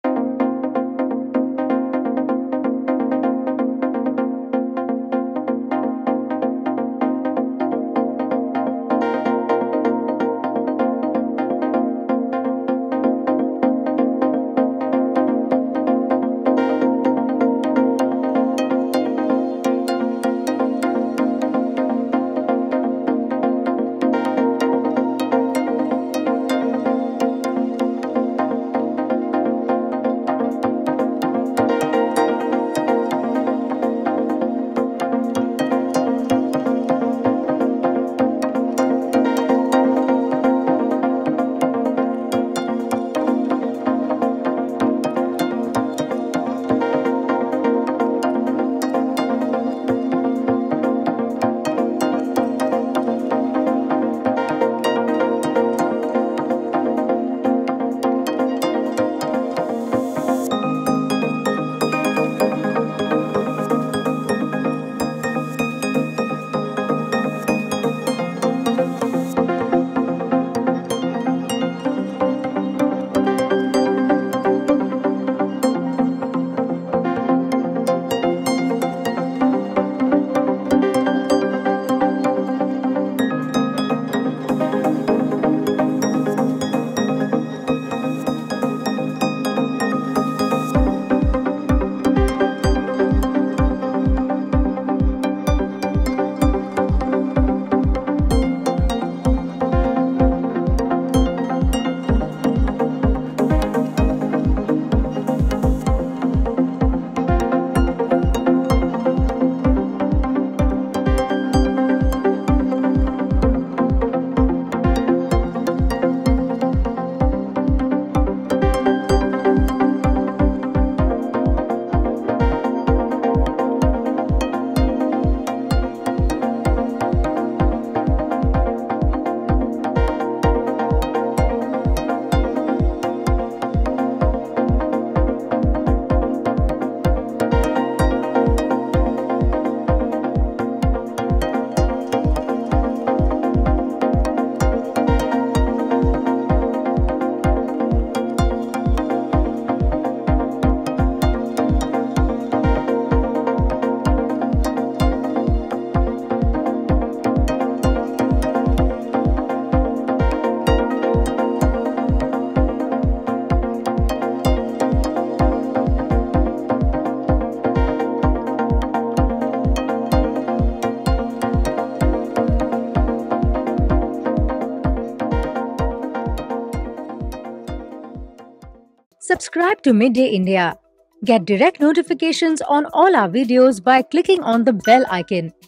Omar Abdullah Praises New Parliament Building Amid Boycott Brouhaha
Despite J&K National Conference being part of the ‘boycott team’, the party’s ‘heavyweight’ Omar Abdullah, hailed the new Parliament building. Taking to his Twitter handle, Omar Abdullah said that the new Parliament building "looks pretty damn impressive". Meanwhile, the political heat is simmering across India over the inauguration of the new Parliament building. Opposition leaders, including Congress, have vehemently opposed PM Modi inaugurating the new Parliament. With a boycott letter, several leaders demanded that President Droupadi Murmu inaugurate the new building. PM Modi will dedicate the newly constructed Parliament building to the nation on May 28.